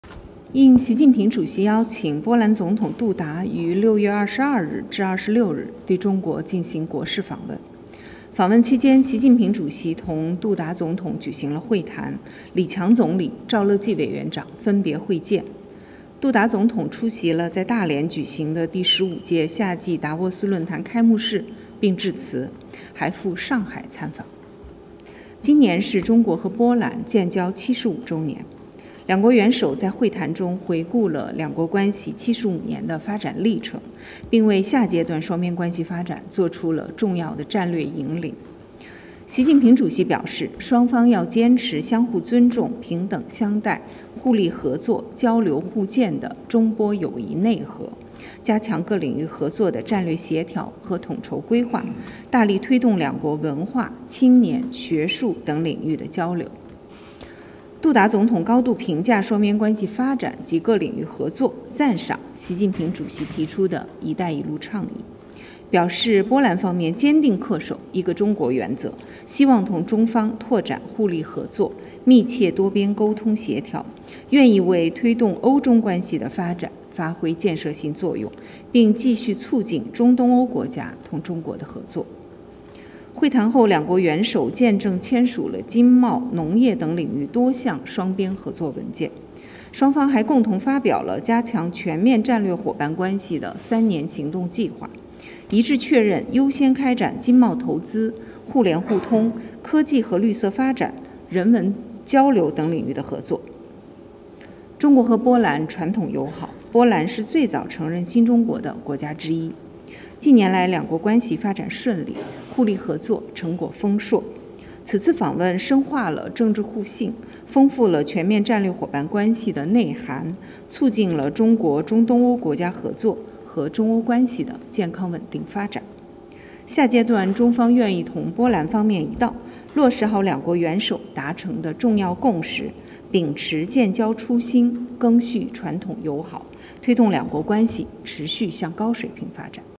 Beijing briefs on President Duda's China visit, says action plan issued for Sino-Polish cooperation
Mao Ning: At the invitation of President Xi Jinping, President of Poland Andrzej Duda paid a state visit to China from June 22 to 26. During the visit, President Xi Jinping held talks with President Duda, and Premier Li Qiang and Chairman Zhao Leji met with him separately. President Duda also attended and addressed the Opening Plenary of the 15th Annual Meeting of the New Champions in Dalian, and visited Shanghai.

This year marks the 75th anniversary of the diplomatic relations between China and Poland. In their talks, the two heads of state reviewed the course of bilateral relations over the past 75 years, and provided strategic guidance for the growth of bilateral ties in the next stage. President Xi Jinping noted that the two sides need to uphold the core spirit of China-Poland friendship, that is, mutual respect, equality, mutually beneficial cooperation, exchanges and mutual learning, step up strategic coordination and overall planning of cooperation in various areas, and put more effort into advancing cultural, youth and academic exchanges. President Duda spoke highly of the growth of bilateral relations and cooperation in various areas, commended the Belt and Road Initiative put forward by President Xi Jinping, reaffirmed Poland’s commitment to the one-China principle, and expressed readiness to expand mutually beneficial cooperation with China, have close communication and coordination with China on multilateral affairs, play a constructive role in advancing the development of Europe-China relations, and continue working for closer ties between Central and Eastern European Countries and China.

After the talks, the two presidents witnessed the signing of several bilateral cooperation documents in areas of economy and trade and agriculture. The two sides also jointly issued the Action Plan on Strengthening the Comprehensive Strategic Partnership (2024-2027), and agreed to give priority to cooperation in areas of economy, trade and investment, connectivity, science, technology and green development, and people-to-people and cultural exchanges.

China and Poland have a traditional friendship. Poland was one of the first countries to recognize the New China. In recent years, China-Poland relations have enjoyed smooth progress, with fruitful outcomes of mutually beneficial cooperation. This visit has deepened the political mutual trust between the two countries, enriched the China-Poland comprehensive strategic partnership, and contributed to the cooperation between China and Central and Eastern European Countries and the sound and steady development of China-Europe relations. Going forward, China is ready to work with Poland to deliver on the important common understandings between the two presidents, stay true to the original aspiration of establishing diplomatic relations, carry forward the traditional friendship, and take the bilateral relations to a higher level.